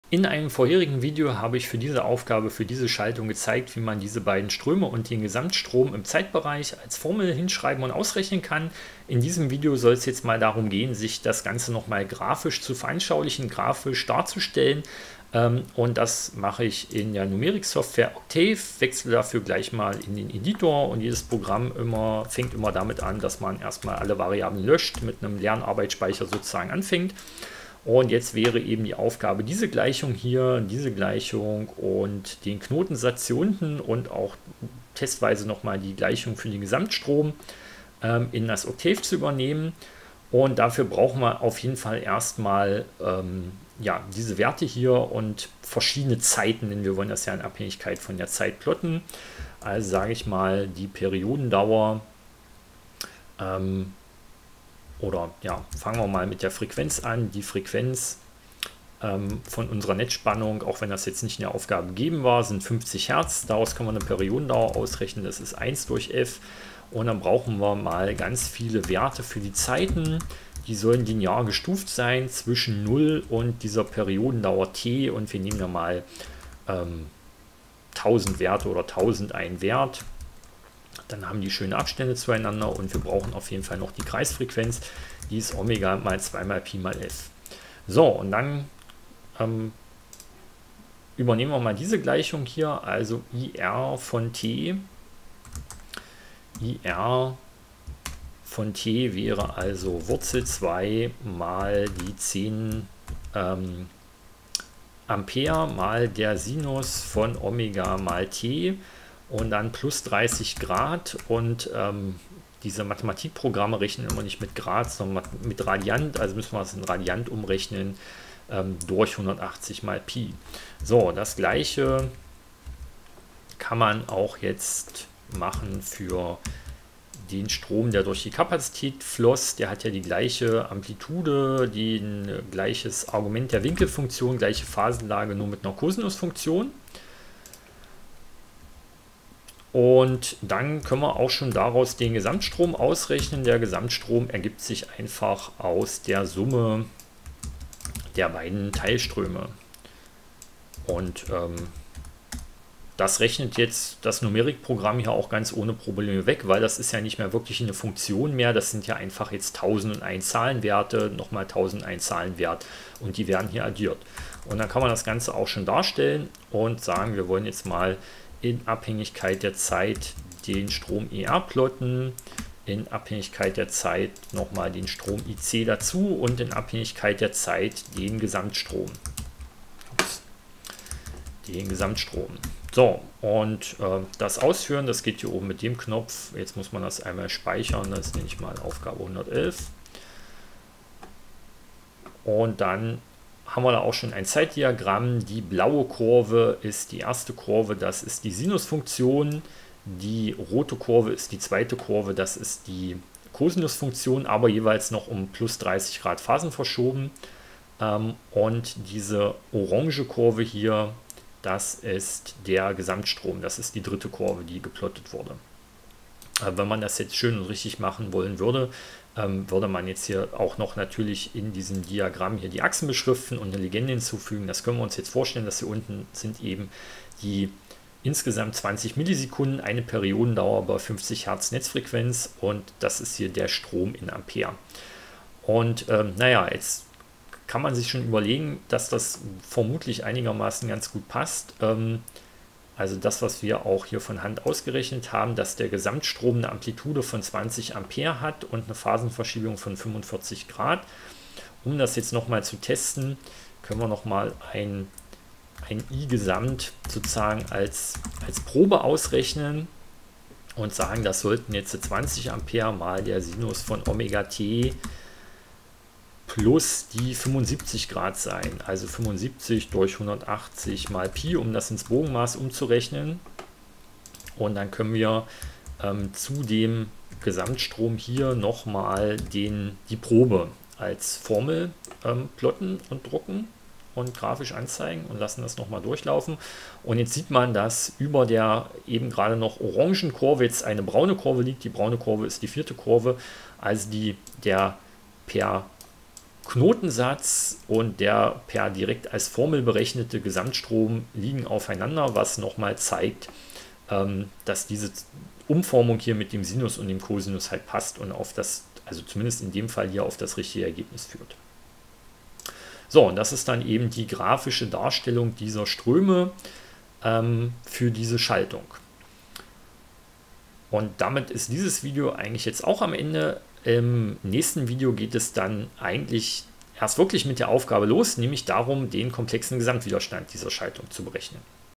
Aufgabe 111 - Schritt 2 - Grafische Darstellung der Teilströme und des Gesamtstromes in Octave
Im zweiten Video dieser Videoserie zur Aufgabe 111 aus unserem Übungsheft zu den Grundlagen der Elektrotechnik mit einer spannungsgespeisten Parallelschaltung aus Widerstand und Kapazität wird gezeigt, wie man die bereits bestimmten Zeitfunktionen der Teilströme durch den Widerstand und die Kapazität sowie den mit Hilfe des Knotensatzes berechneten Gesamtstrom in der Numeriksoftware GNU Octave (bzw. in MATLAB) grafisch darstellt.